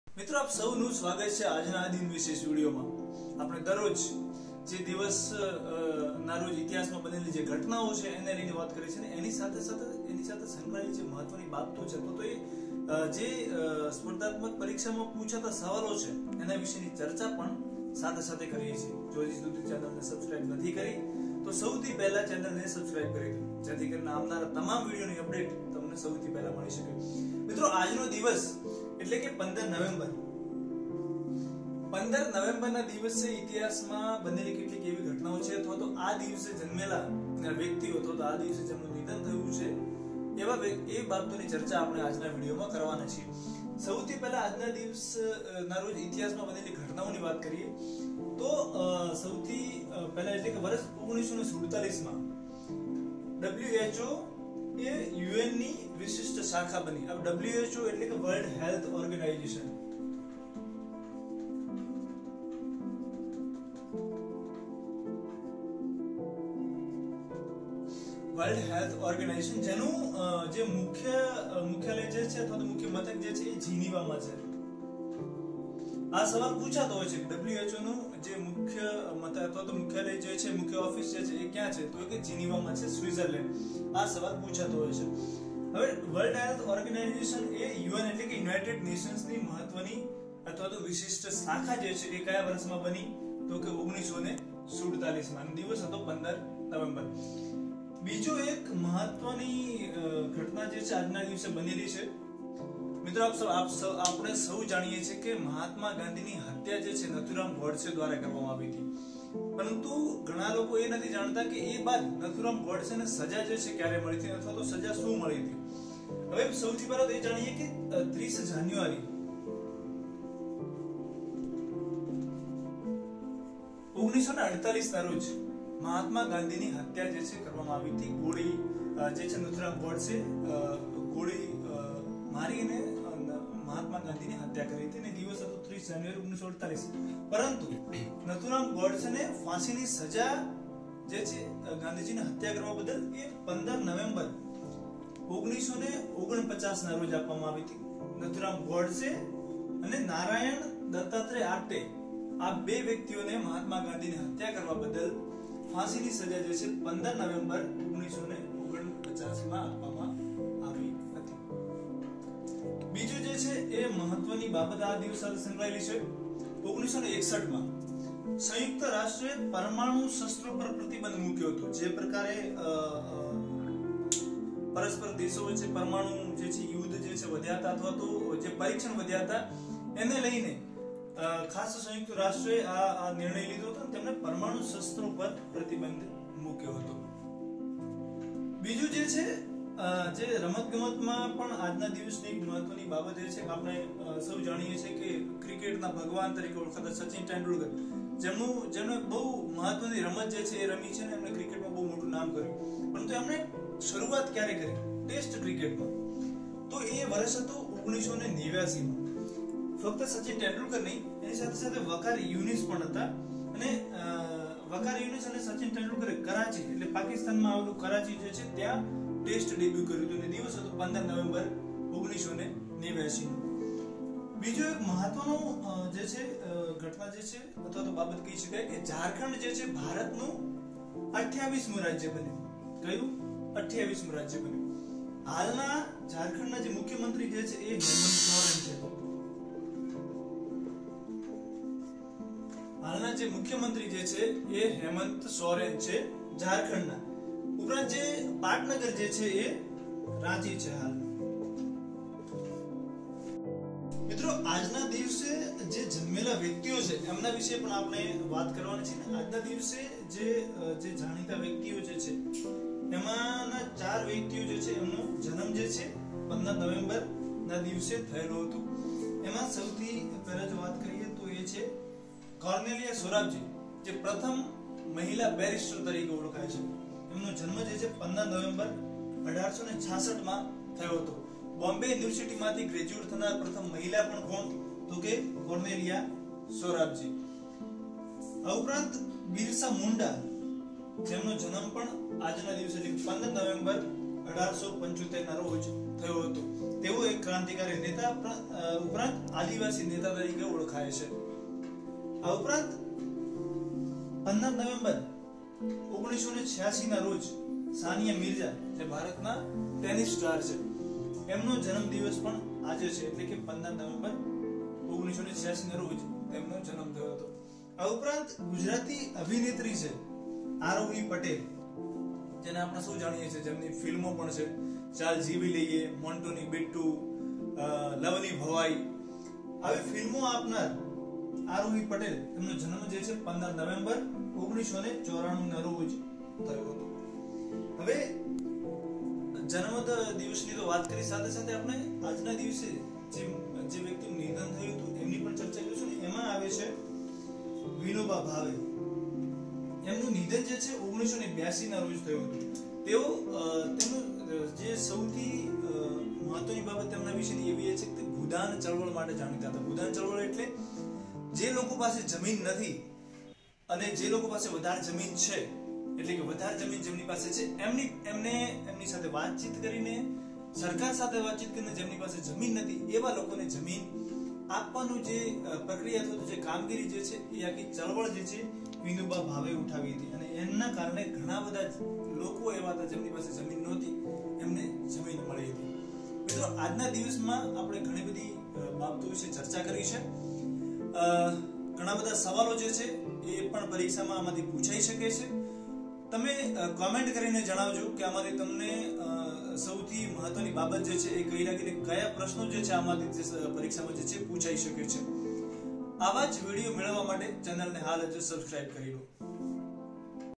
15 નવેમ્બર દિન વિશેષ | 15 November Day Special | Education | Nathuram Godse
1947 - વર્લ્ડ હેલ્થ ઓર્ગેનાઈઝેશન યુનાઈટેડ નેશન્સની વિશિષ્ટ શાખા બની.
1949 - મહાત્માગાંધીની હત્યારા નાથુરામગોડસે અને નારાયણ દત્તાત્રેય આપ્ટેને ફાંસી આપવામાં આવી.
1961 - સંયુક્ત રાષ્ટ્રએ પરમાણુશસ્ત્રો પર પ્રતિબંધ મૂક્યો હતો.
1989 - વકાર યુનિસ અને સચિનતેંડુલકરે કરાચી, પાકિસ્તાનમાં ટેસ્ટ ડેબ્યુ કર્યું.
2000 - ઝારખંડ ભારતનું 28મું રાજ્ય બન્યું.

જન્મ
1866 - ભારતની પ્રથમ મહિલા બેરિસ્ટર કોર્નેલિયાસોરાબજીનો જન્મ થયો હતો.
1875 - ભારતના પ્રખ્યાત સ્વાતંત્ર્ય સેનાની, આદિવાસી નેતા, બિરસામુંડાનો જન્મ માં થયો હતો.
1986 - પ્રખ્યાત ભારતીય ટેનિસ ખેલાડી સાનિયામિર્ઝાનો જન્મ થયો હતો.
1994 - ગુજરાતી ફિલ્મ અભિનેત્રી આરોહીપટેલનો જન્મ થયો હતો

નિધન
1982 - ભૂદાન ચળવળના પ્રણેતા વિનોબાભાવેનું અવસાન થયું.

1947 - The WorldHealthOrganization becomes a special branch of the UnitedNations.
1949 - Nathuram Godse and NarayanDattatreyaApte, assassins of MahatmaGandhi, are hanged.
1961 - The United Nations bans nuclear weapons.
1989 - WaqarYounis and SachinTendulkar make their Test debuts in Karachi, Pakistan.
2000 - Jharkhand becomes the 28th state of India.

Born
1866 - India's first female barrister Cornelia Sorabjee is born.
1875 - BirsaMunda, a prominent Indian freedom fighter and tribal leader, is born.
1986 - Famous Indian tennis player SaniaMirza is born.
1994 - Gujarati film actress AarohiPatel is born

Died
1982 - VinobaBhave, pioneer of the Bhudan movement, dies.